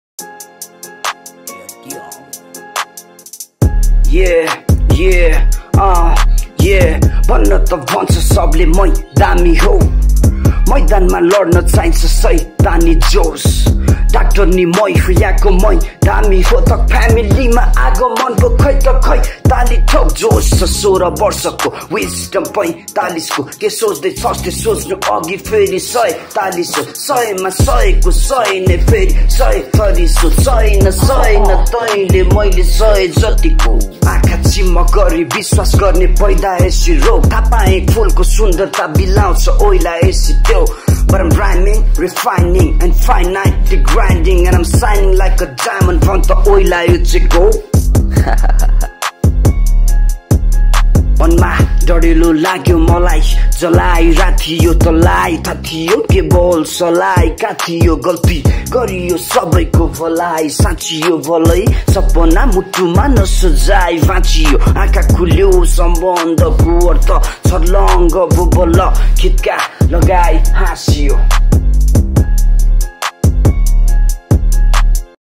Anonymous
1. Name: Bipin Subedi
2. Stage name: अज्ञात (Anonymous)
3. Age: 28
4. Artist: Not signed to any record label , emerging artist

5. Address: Jadibuti, Kathmandu